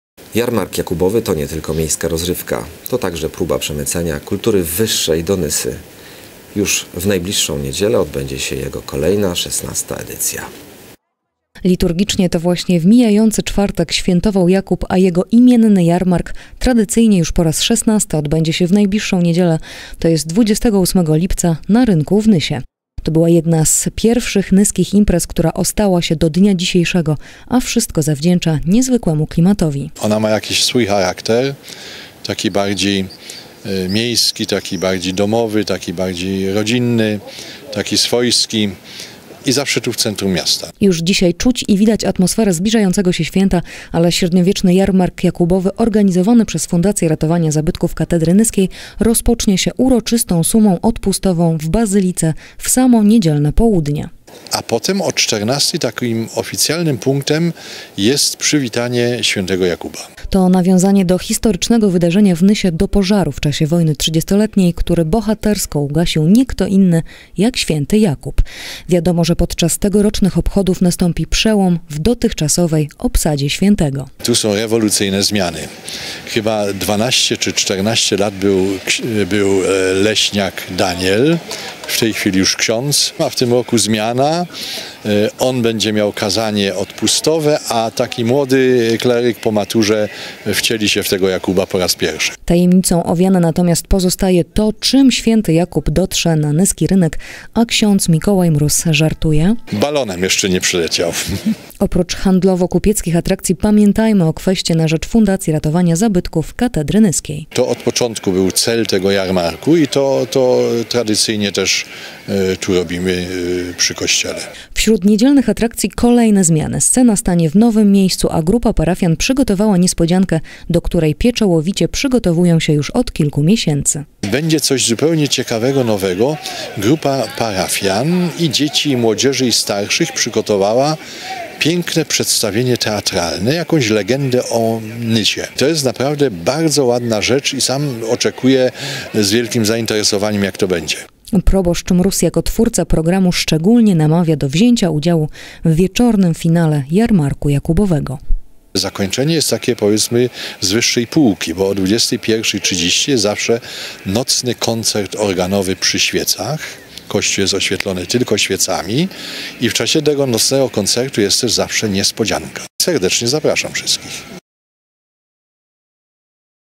Nowa odsłona Jarmarku Jakubowego
Jarmark Jakubowy to nie tylko miejska rozrywka, to także próba przemycania kultury wyższej w Nysie. Już w najbliższą niedzielę odbędzie się jego kolejna, XVI edycja.